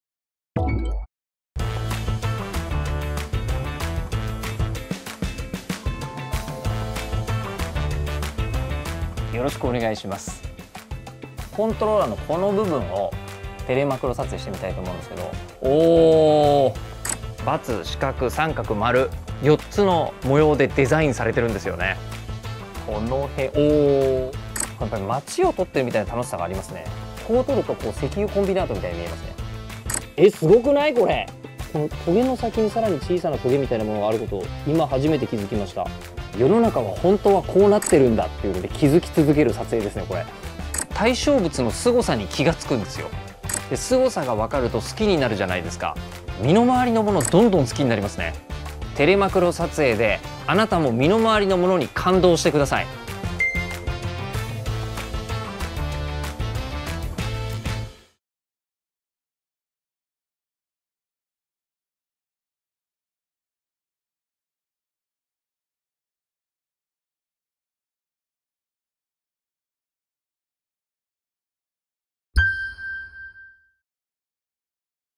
​Xperia 1 VI | Telephoto macro – "Unveiling a Tiny World" – Yoshida Hisanori​
The new ONE: Xperia1VI – Zoom into wonder. ​

(設定から日本語字幕を表示できます）​​
*Multiple language subtitles available from settings:​​
(日本語/English/Deutsch/繁体字)​​

Discover a whole new world up close, beyond what the eye can see! 👀✨​

Join Mr. Hisanori Yoshida as he dives into the Telephoto macro feature on the Xperia1VI. 📸 ​

This amazing mode lets you capture small details in a big, bold way - revealing textures that are invisible to the naked eye.​

With the ability to shoot just 4cm away from your subject, what cool things will you capture in Telephoto macro?​

If you feel inspired, why not show us your Telephoto macro photos by tagging your best shots with WowXperia on Instagram!​​